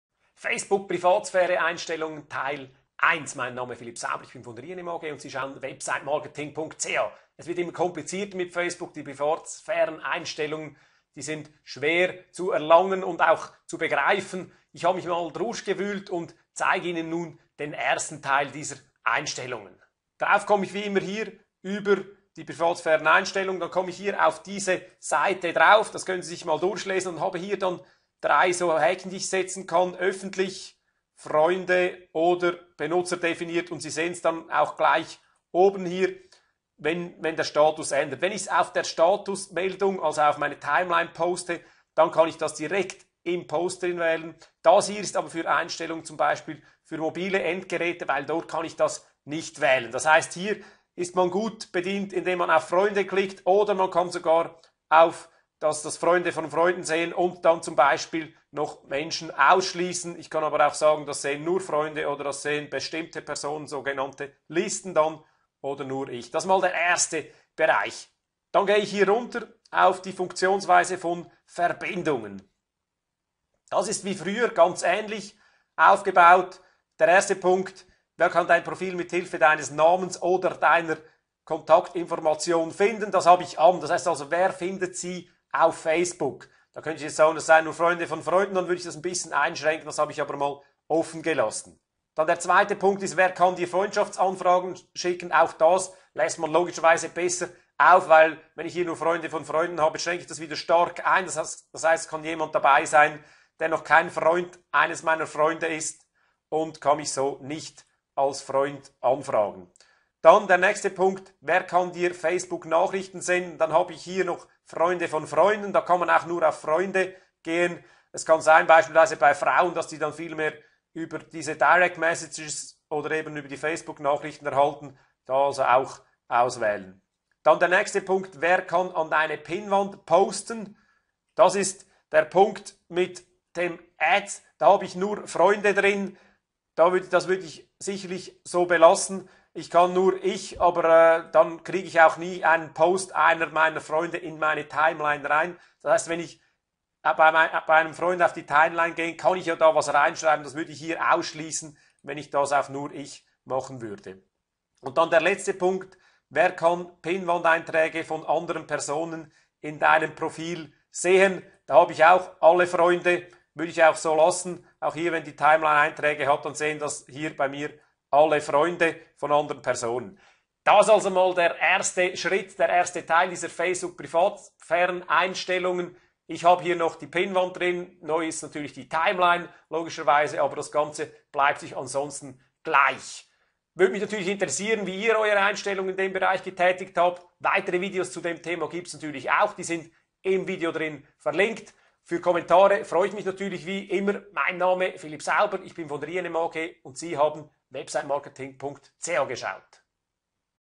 Facebook Privatsphäre Einstellungen und Datenschutz - Verbindungen (Teil1/3)
- Teil 1: die Verbindungen zu andern Personen auf Facebook.
Facebook regiert die Social Networks, hat mehr Mitglieder als jede andere Plattform und verbreitet Inhalte weit über die Seite hinaus. Wie ich mich davor schützen kann, dass nur meine Freunde oder die Freunde meiner Freunde meine Inhalte wie z.B. Bilder sehen können, kann man unter den Datenschutz & Privatsphären Einstellungen angeben. Kennen Sie die Facebook Privatsphäre? Wenn nicht, dann unbedingt reinschauen und Inhalte schützen.

Philipp Sauber
INM AG